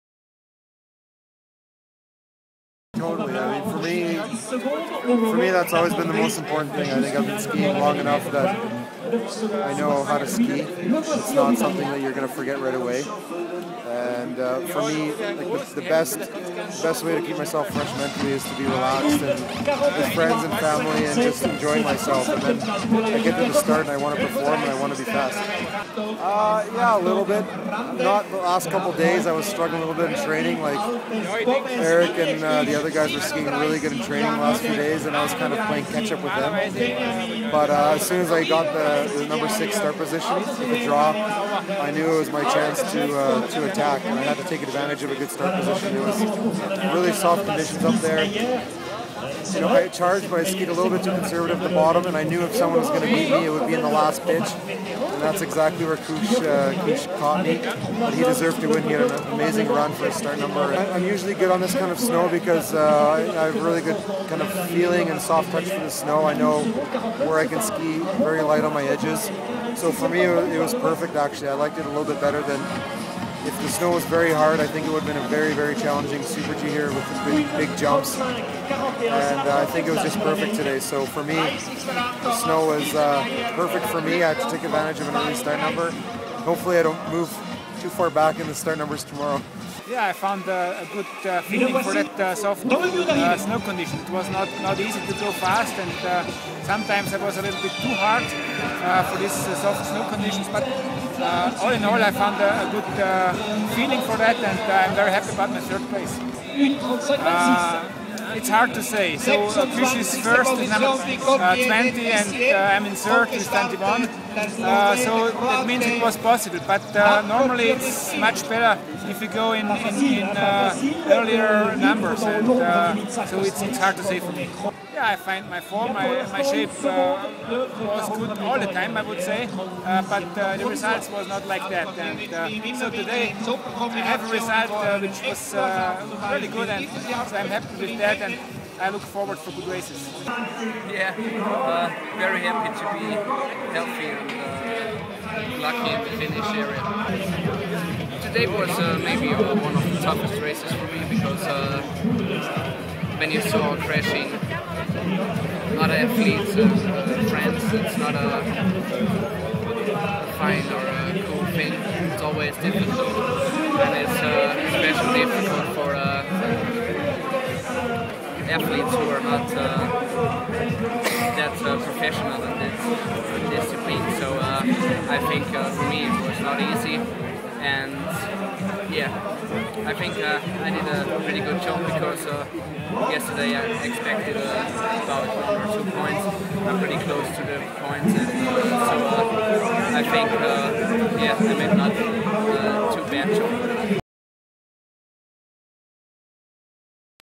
Crans Montana, Super G comments
Jan Hudec, Benjamin Raich and Marcel Hirscher talk about today's Super G race